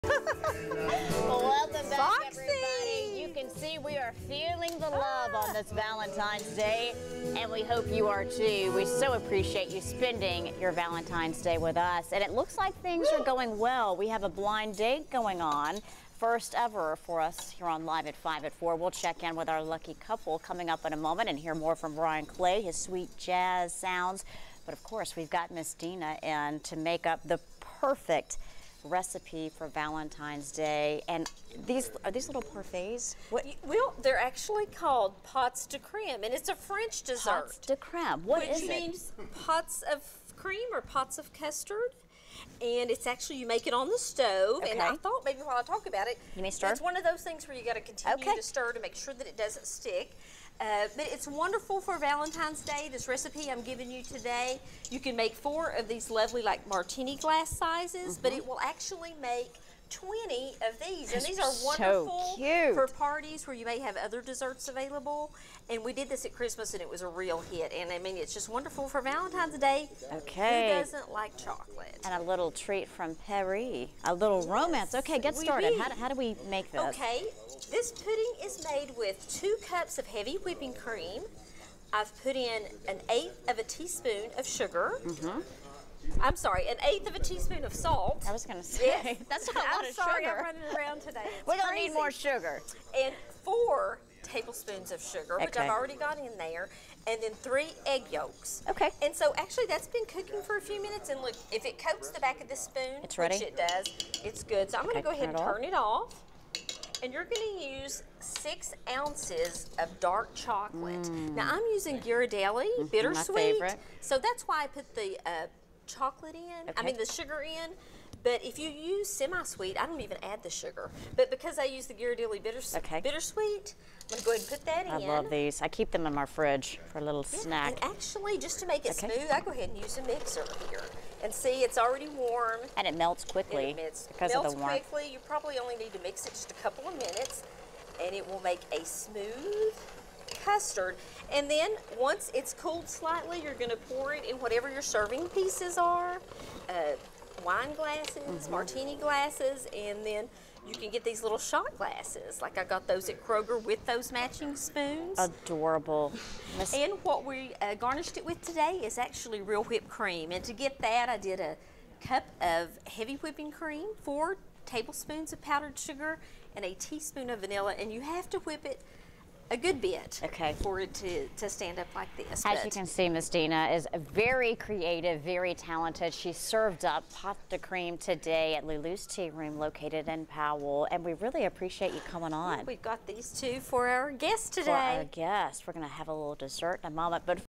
This is the dessert you've been wanting for Valentine's Day (but may have never heard of)
Deana Hurd with Lulu's Tea Room prepares Pot de creme. Lulu's Tea Room is located in Powell, lulustearoom.com. Feb. 14, 2019-4pm